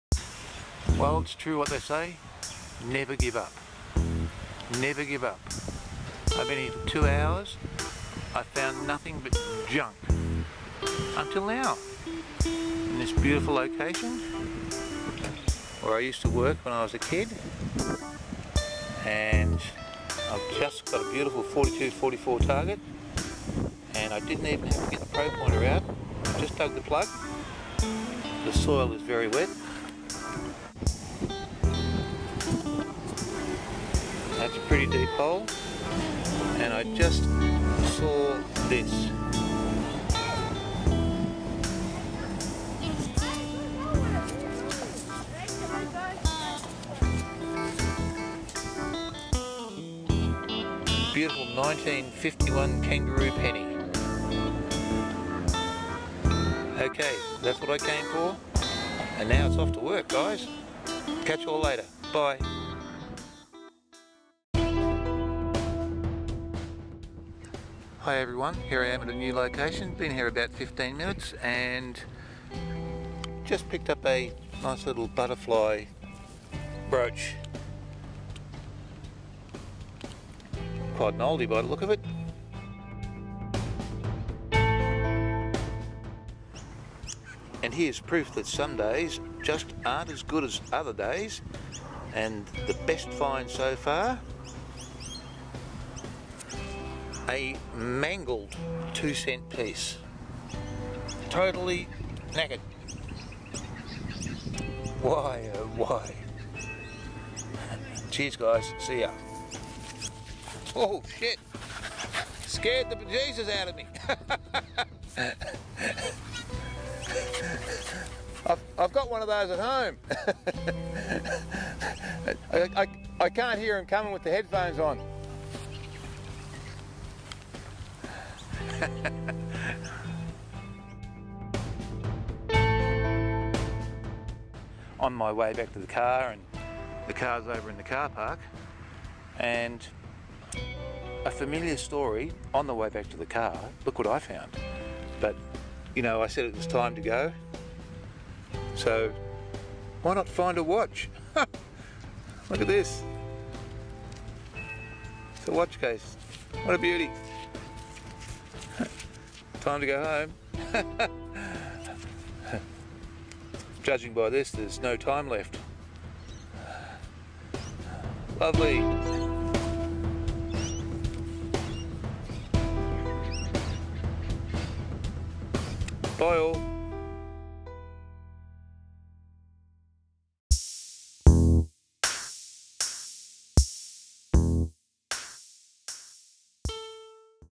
Park Detecting - "When Puppies Attack"
Hi all, went detecting...got pounced on by a couple of Puppies. Woof!  Enjoy.